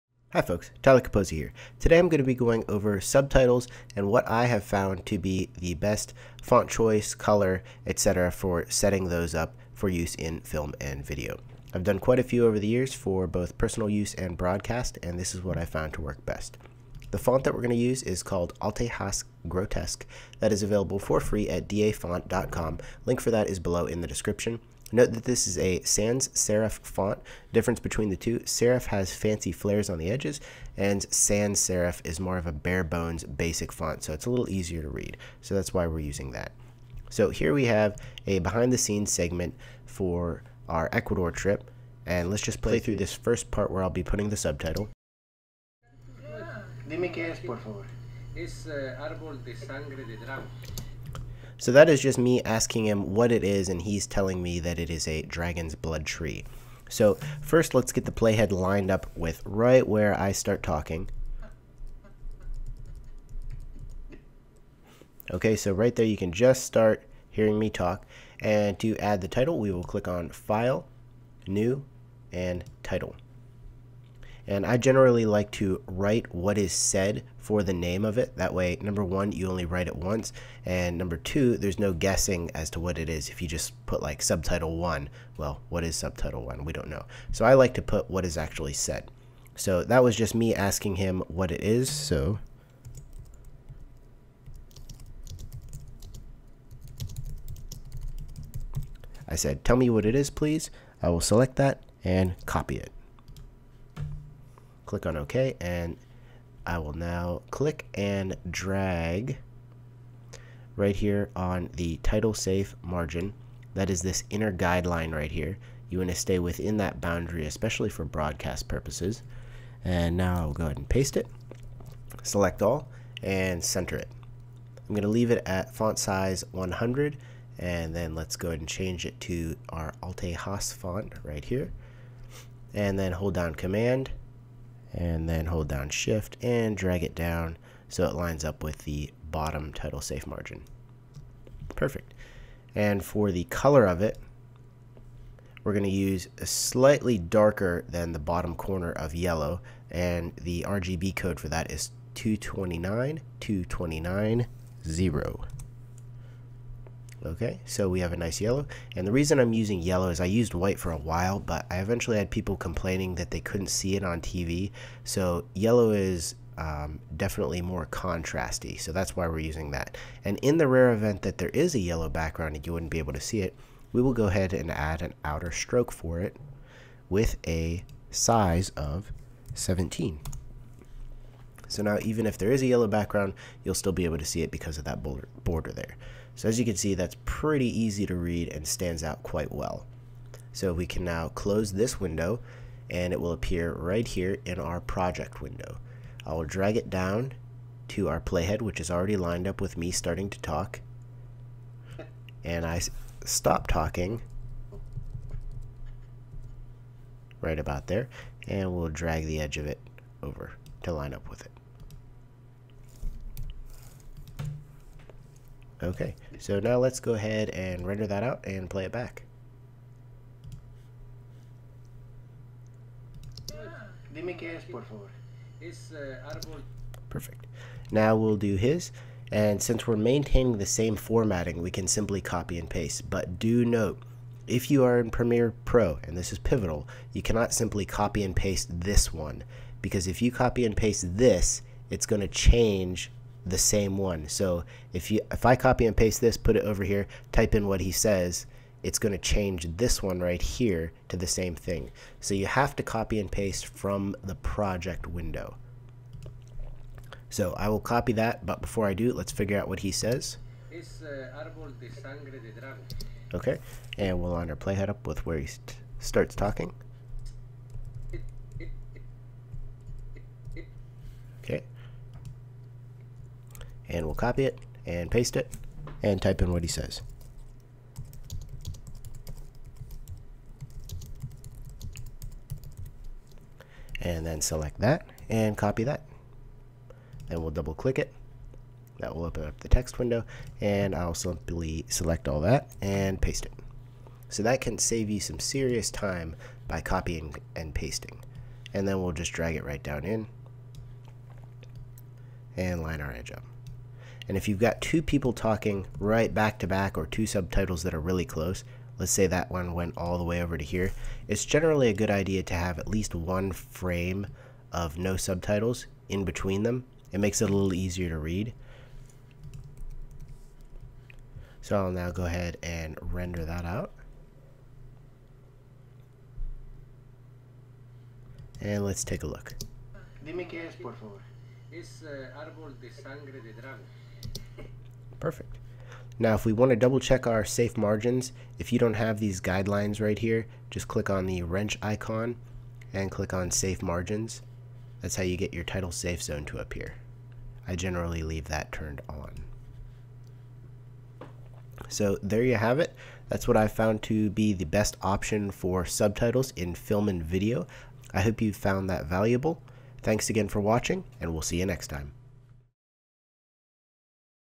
Subtitles in Premiere Pro
This tutorial covers what I have found to be the best choice for subtitles for video in Premiere Pro. What do you use? I'd love to hear.